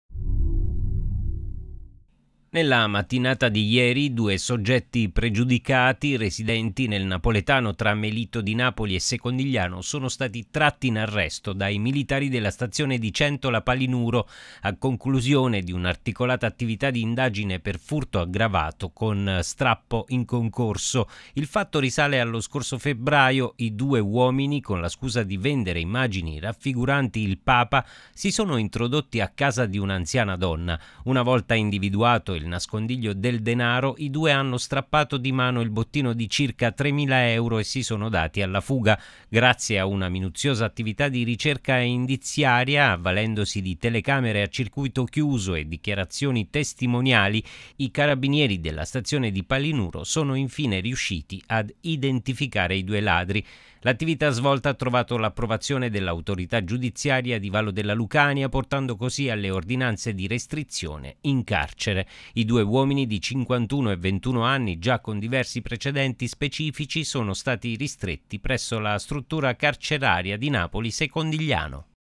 Palinuro, Arrestati due ladri che ingannavano i malcapitati con le immagini del Papa.
Nella mattinata odierna due soggetti pregiudicati residenti nel napoletano tra Melito di Napoli e Secondigliano, sono stati tratti in arresto dai militari della Stazione di Centola Palinuro a conclusione di un’articolata attività di indagine per furto aggravato con strappo in concorso.